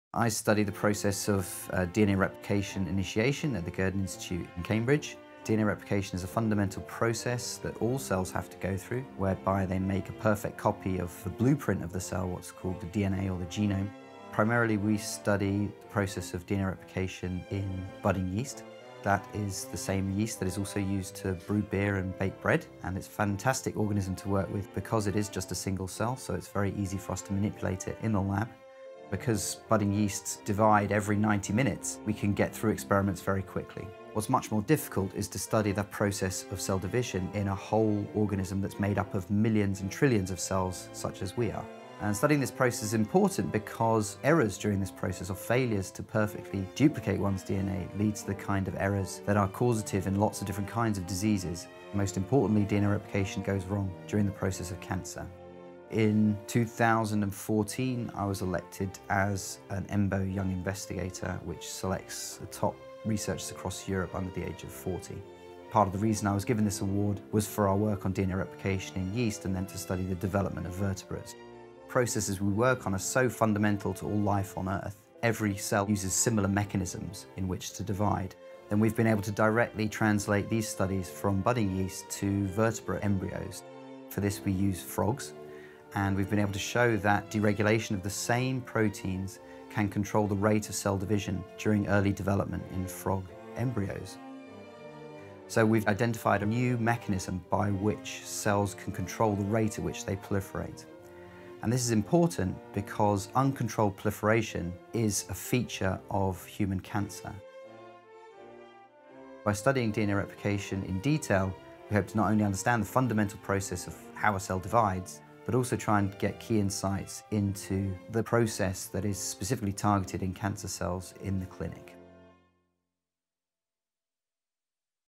Gurdon Institute | Meet Phil Zegerman
Come inside the Wellcome Trust/Cancer Research UK Gurdon Institute, University of Cambridge, to meet group leader Dr Phil Zegerman, as he describes his research on DNA replication. In 2016 the Gurdon Institute celebrates 25 years of studying development to understand disease.